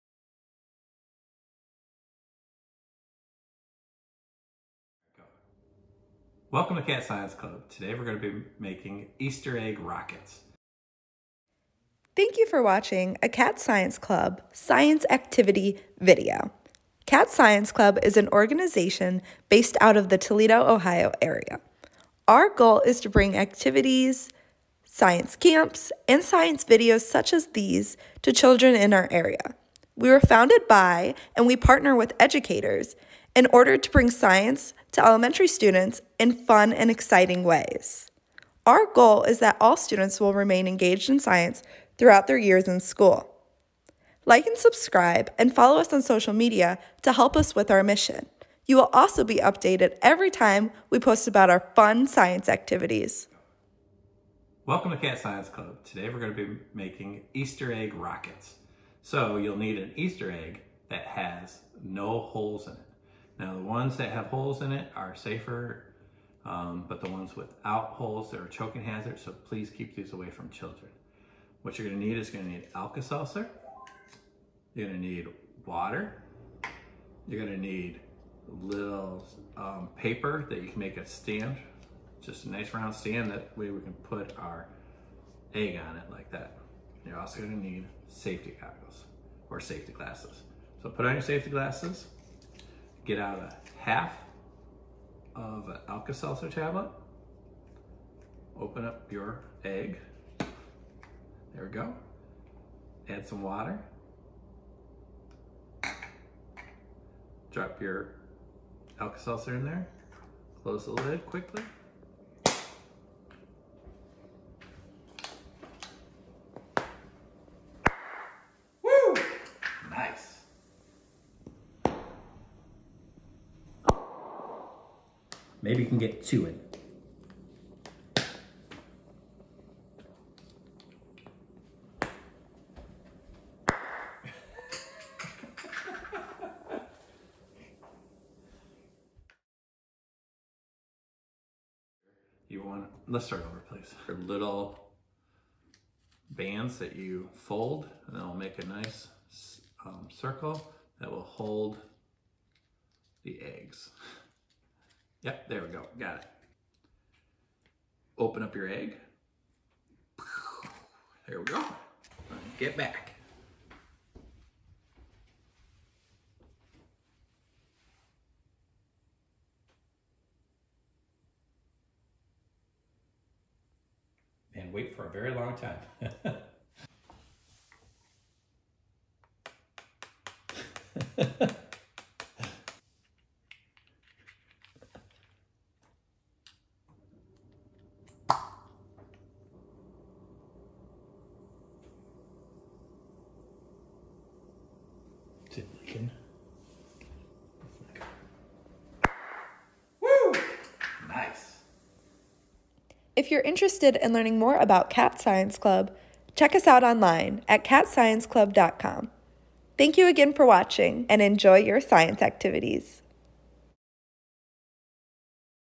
Build Your Own Easter Egg Rocket at Home!
Build Your Own Easter Egg Rocket at Home! Have fun and learn about science.

This is a great activity to do while at home after Easter. You will need the old plastic Easter Eggs that have no holes in them. We found some from Grandma Cat.

BE CAREFUL and keep the eggs away from children. The holes are in there for a good reason.